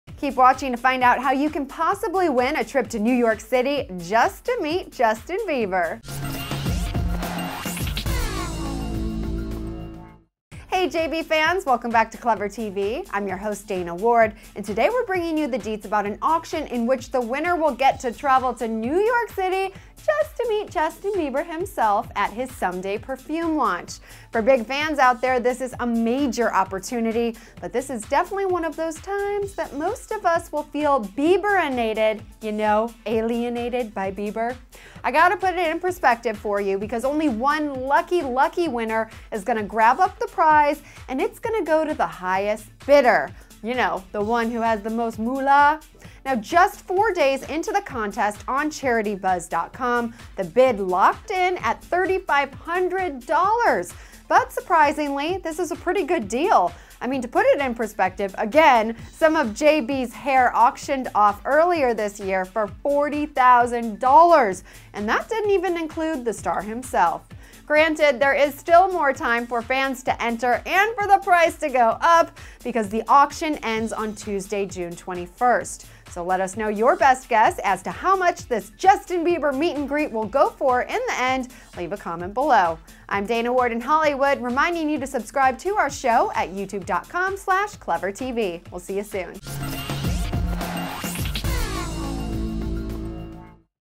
Charity Auction Gives Fan Chance To Meet Justin Bieber
Keep watching to find out how you can possibly win a trip to New York City to meet Justin Bieber.

Hey JB fans! Welcome back to ClevverTV. I'm your host Dana Ward and today we're bringing you the deets about an auction in which the winner will get to travel to New York City to meet Justin Bieber himself at his Someday perfume launch. For big fans out there, this is a major opportunity, but this is definitely one of those times that most of us will feel bieber-enated -- ya know alienated by Bieber. To put it in perspective, one lucky lucky winner will grab up the prize, but it will go to the highest bidder. Just 4 days into the contest on CharityBuzz.com, the bid locked in at 3500.00. But surprisingly, this is a pretty good deal -- just to put it in perspective, some of JB's hair auctioned off for 40-thousand dollars -- and that didn't even include the star himself. Granted, there is still more time for fans to enter the auction because it ends on Tuesday June 21.